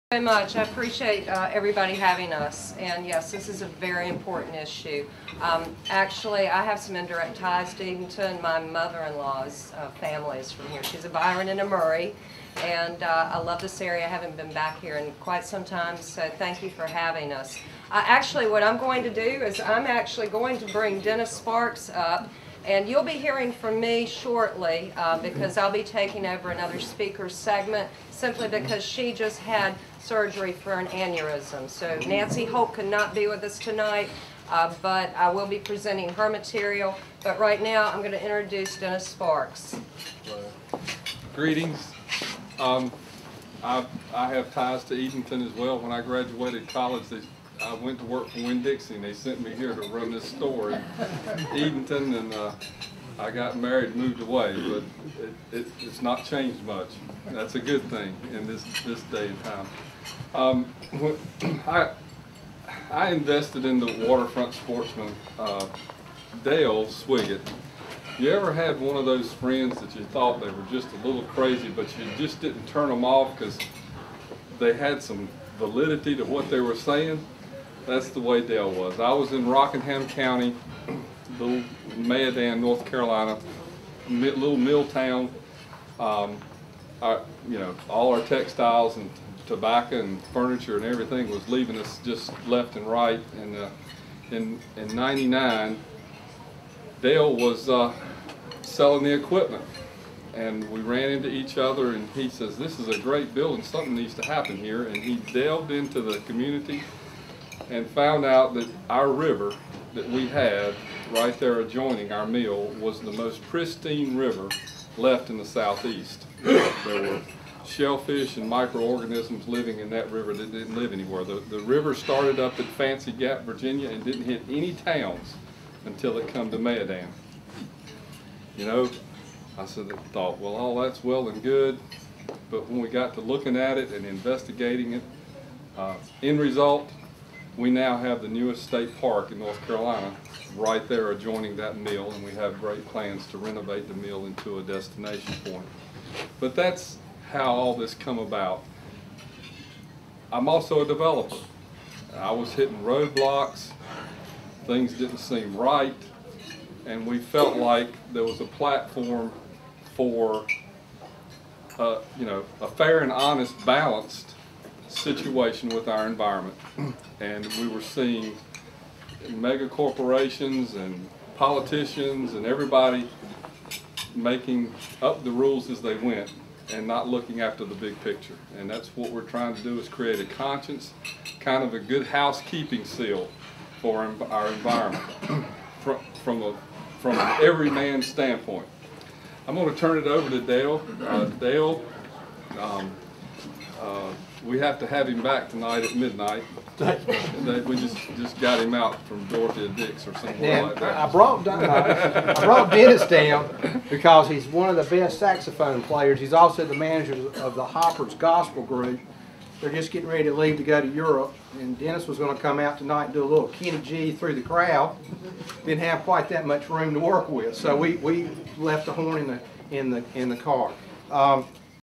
Dennis Sparks intro
Dennis Sparks introduces everyone at the Edenton meeting.